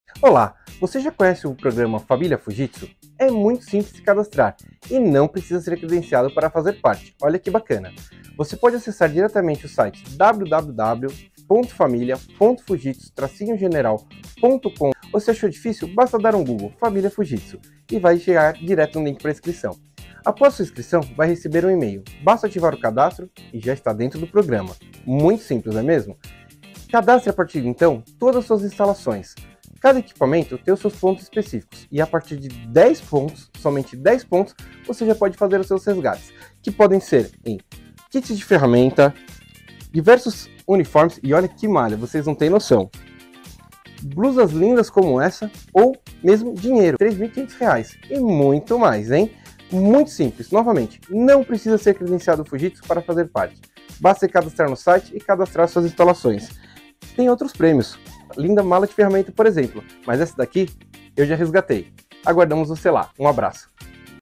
Programa Familia Fujitsu
Faaaala  Refrigerista,

Você já conhece o Programa Família Fujitsu?

Com ainda mais benefícios e oportunidades de crescimento, o instalador parceiro poderá contar com um sistema novo, prático e intuitivo, além de vantagens exclusivas que só nosso programa de fidelidade pode oferecer.Para participar é muito simples, rápido e prático.

Acesse O que antes já era bom, agora ficou ainda melhor.